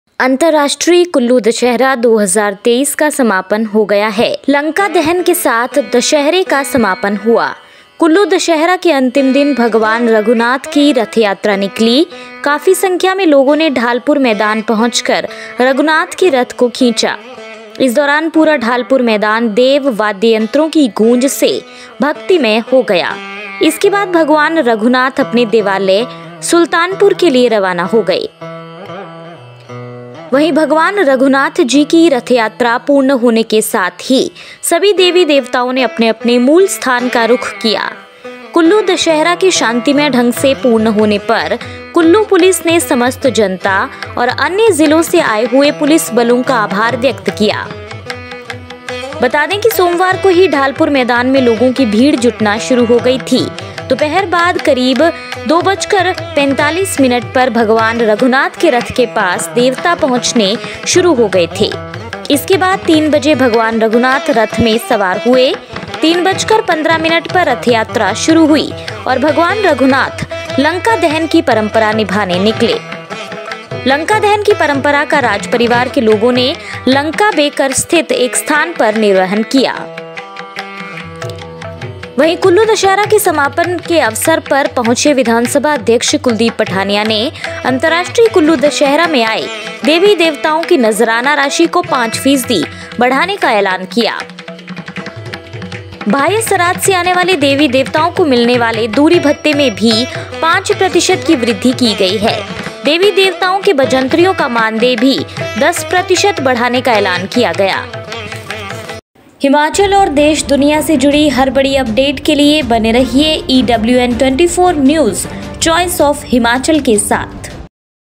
कुल्लू दशहरा : ढालपुर मैदान में उमड़ा भक्तों का सैलाब,लंका दहन के साथ हुआ समापन
International Kullu Dussehra-2023 has concluded. Dussehra was equated with burning of Lanka. Rath Yatra of Lord Raghunath took place on the last day of Kullu Dussehra. A large number of people reached Dhalpur ground and pulled Raghunath's chariot. During this time, the entire Dhalpur ground became devotional with the echo of musical instruments. After this Lord Raghunath left for his temple Sultanpur.
At the same time, with the completion of Lord Raghunath ji's Rath Yatra, all the Gods and Goddesses turned to their respective native places. On the peaceful completion of Kullu Dussehra, Kullu Police expressed gratitude to the entire public and the police forces coming from other districts.